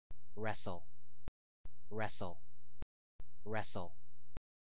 How to Say or Pronounce Wrestle
This video shows you how to say or pronounce Wrestle.

How accurate does it say Wrestle? How would you say Wrestle?